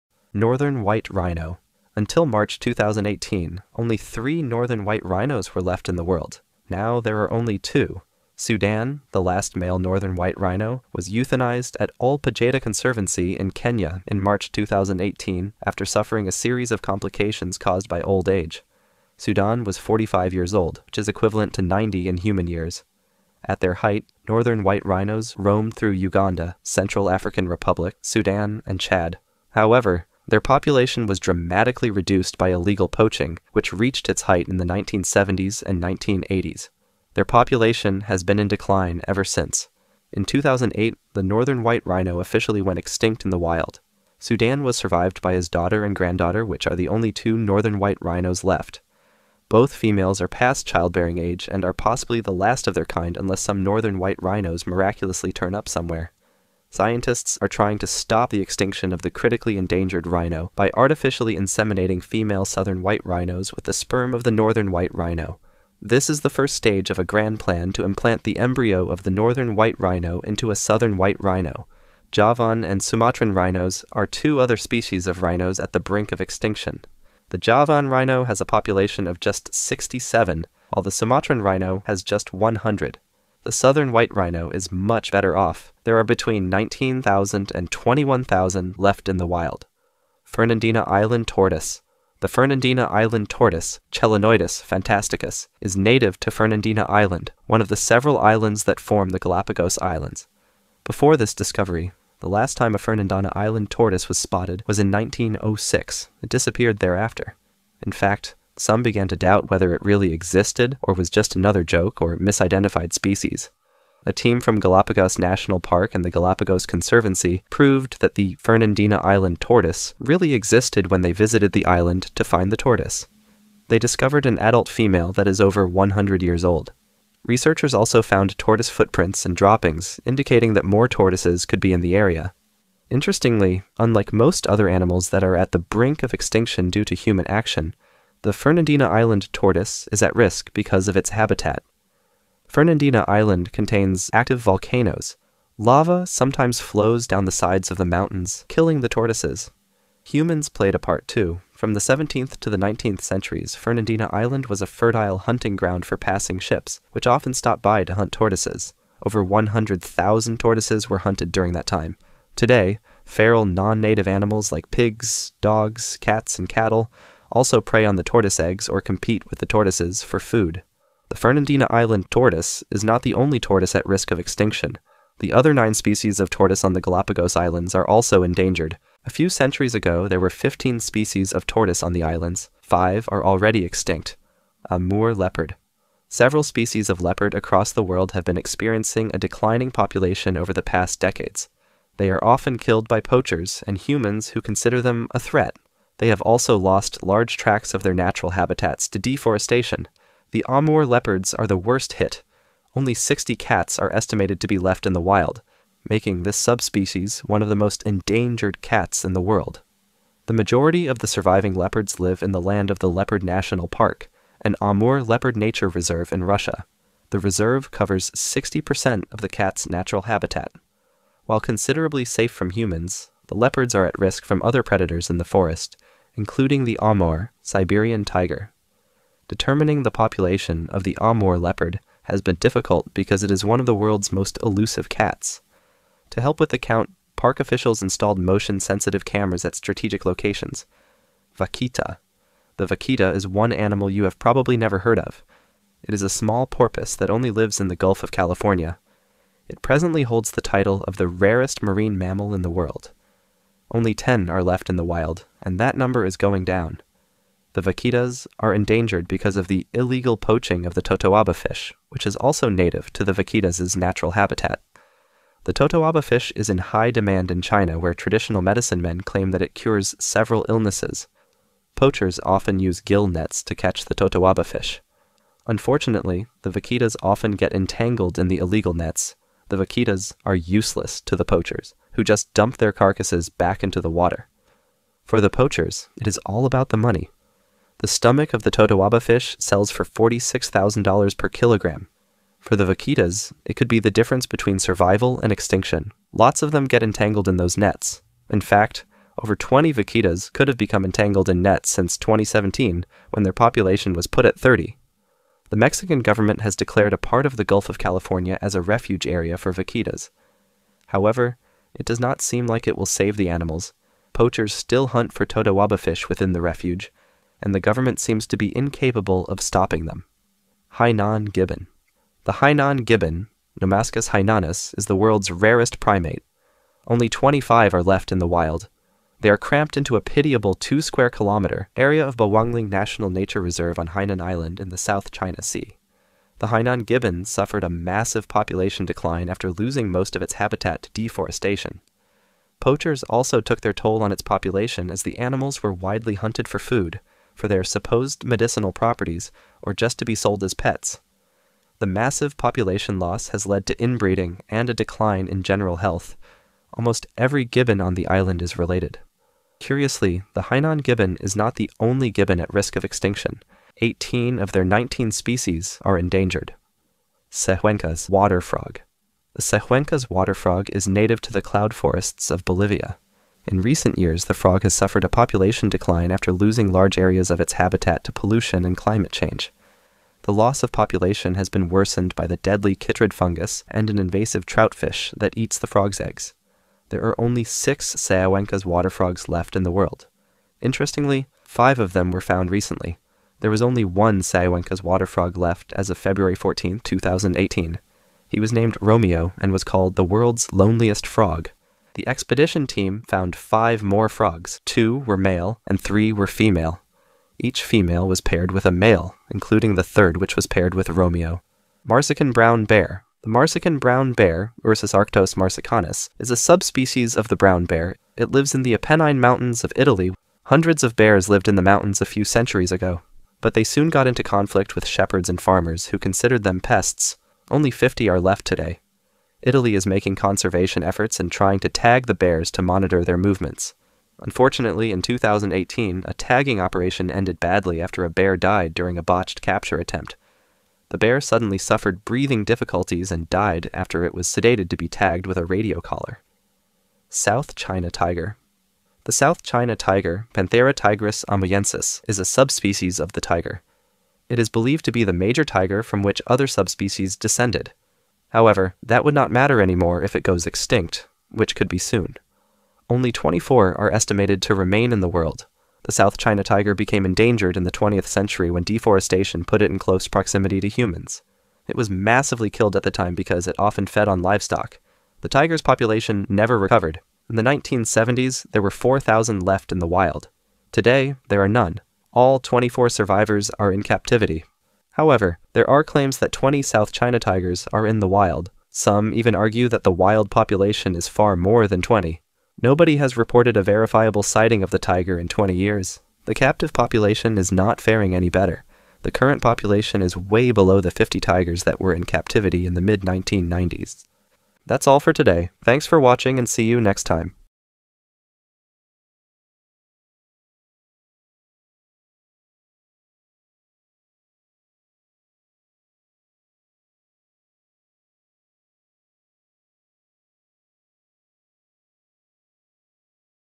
Endangered Animals That Could Become Extinct In A Few Years
Some animals are on the verge of extinction.Some have even gone extinct in the wild and are only around because we had them in captivity.

0:00 - Northern White Rhino
1:30 - Fernandina Island Tortoise
3:04 - Amur Leopard
4:01 - Vaquita
5:20 - Hainan Gibbon
6:07 - Sehuencas Water Frog
6:51 - Marsican Brown Bear
7:27 - South China Tiger